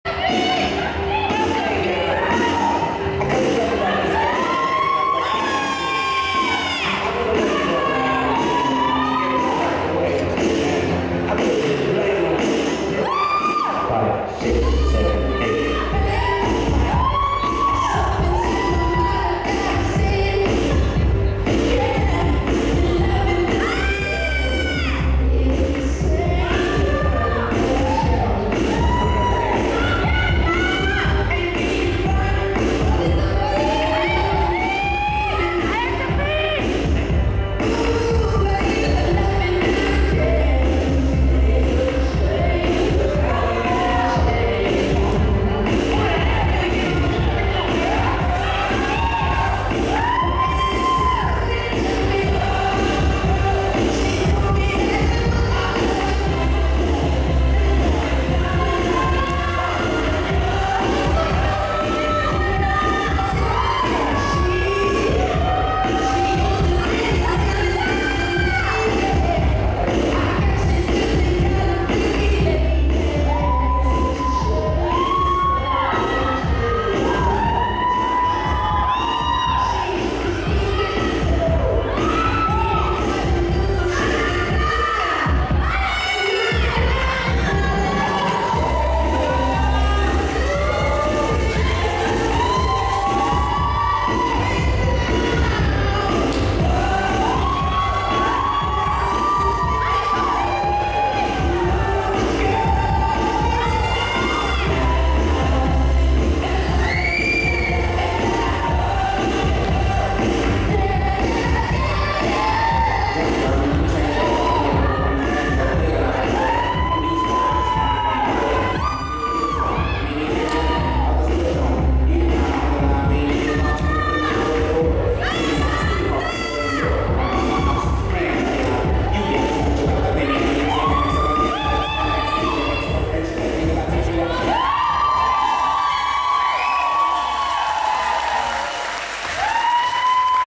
WCDF German Summer Classics 2013 - Adult Intermediate
Dance D: Cuban
1st Place overall in Adult Intermediate Female